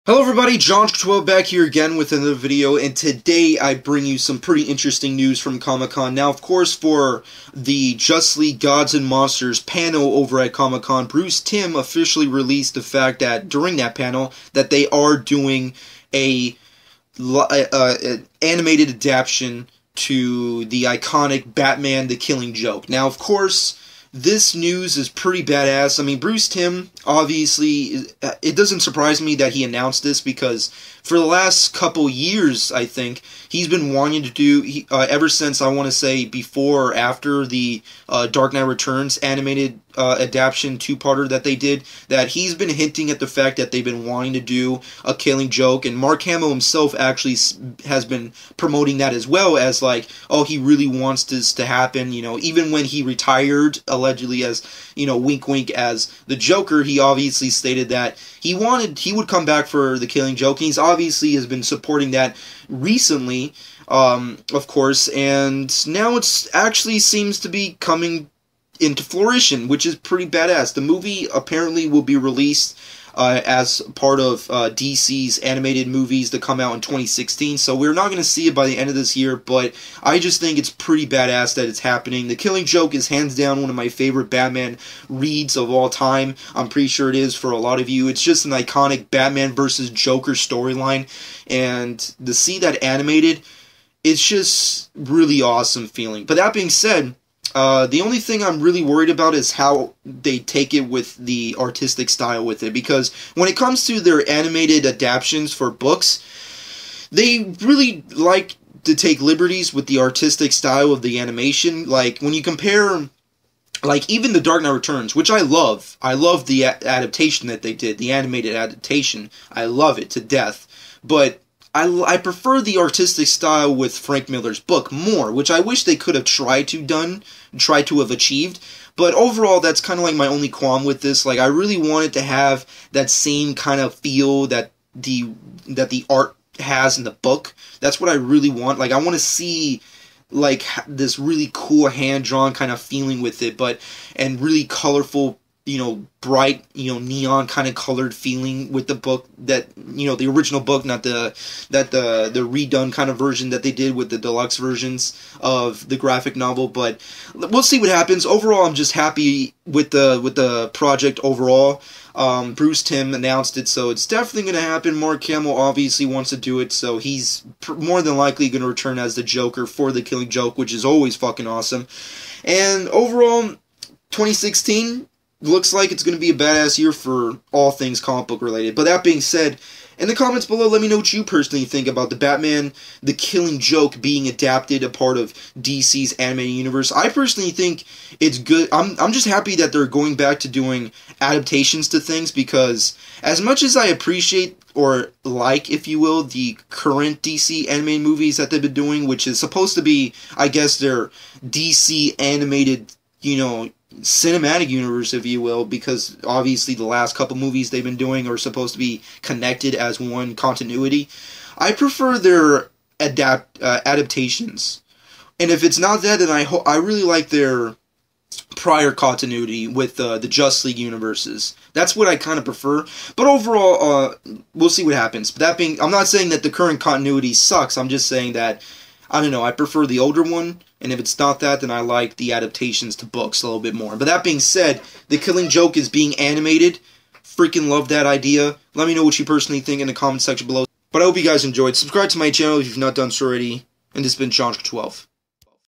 The Killing Joke animated coming 2016
Hello everyone jonjoker12 back again with another video for today.
Today i give you my personal thoughts on batman the killing joke becoming an animated feature.